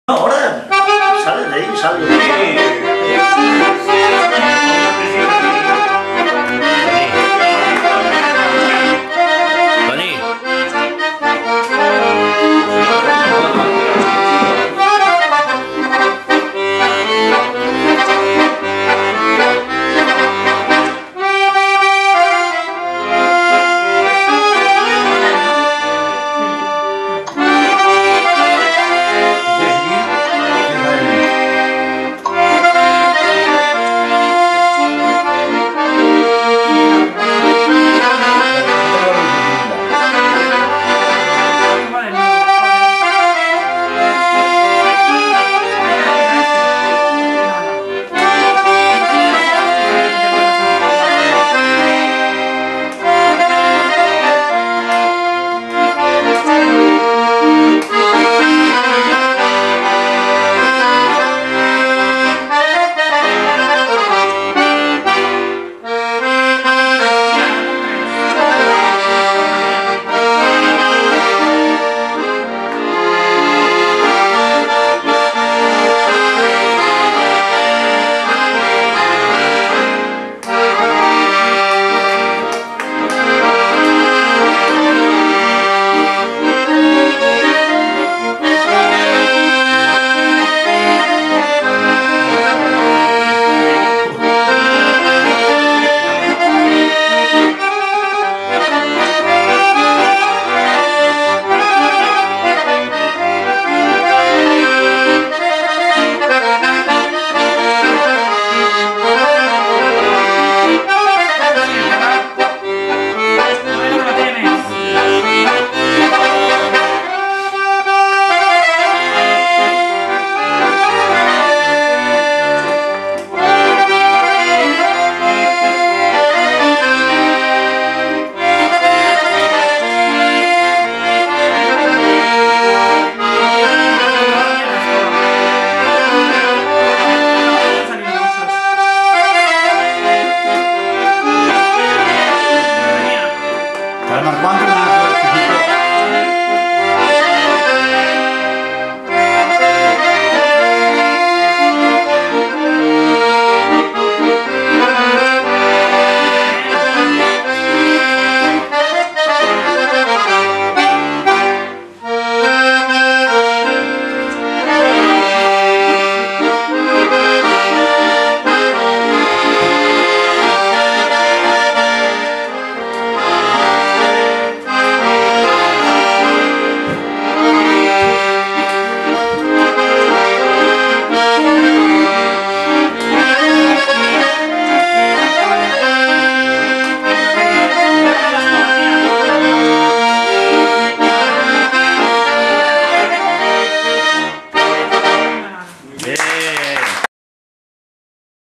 Francisco tocando Scandalle de Loureiro
cancion tocada por francisco con acordeon scandalle de la familia loureiro.